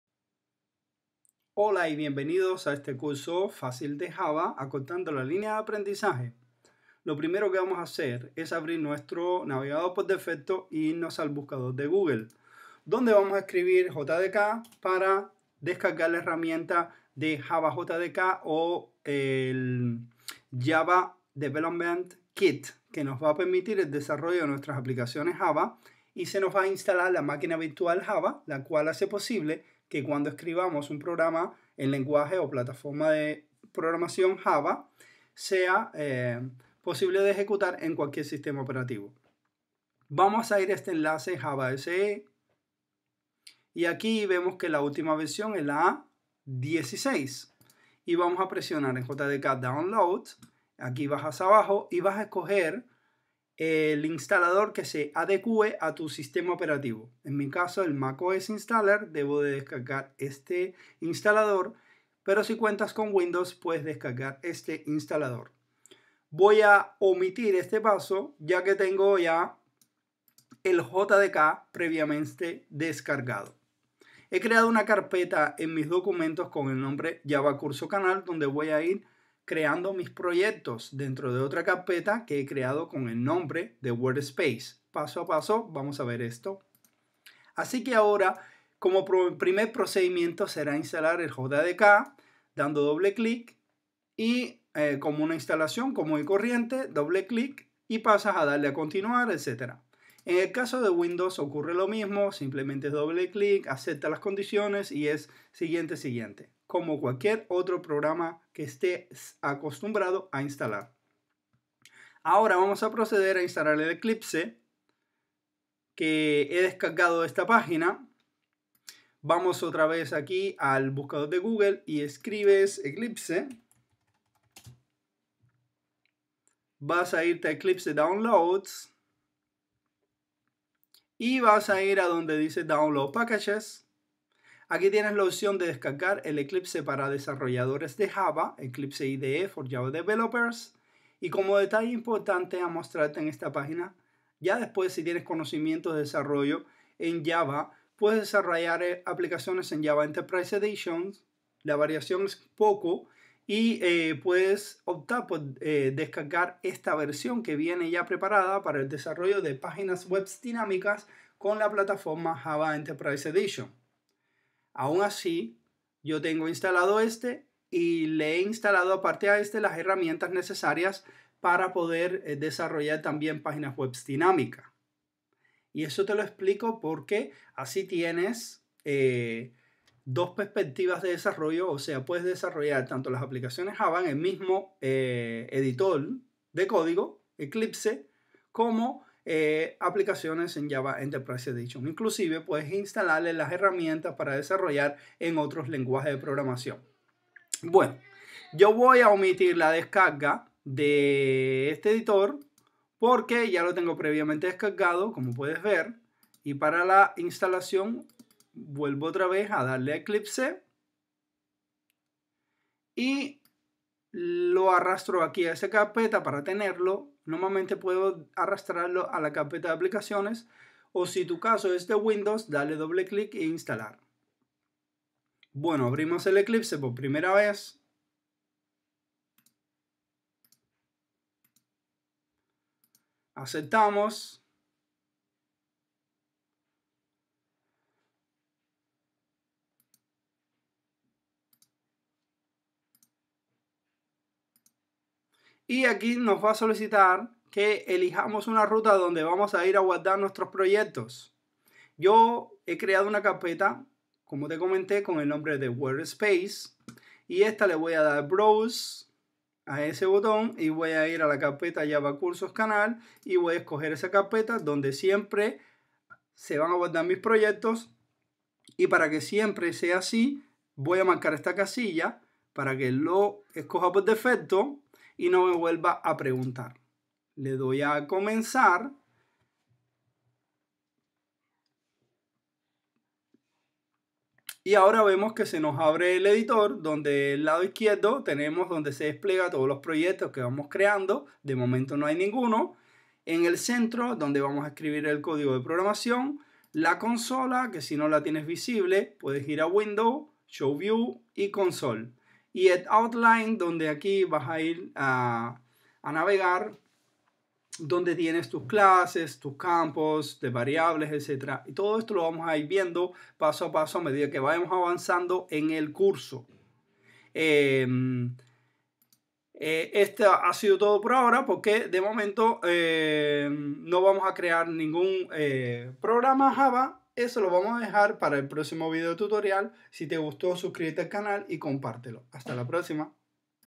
Curso Java facil instalar las herramientas de desarrollo JDK y Eclipse
Curso de Java fácil y rápido, acortando la línea de aprendizaje.
Hoy vamos a instalar las herramientas de desarrollo que necesitamos.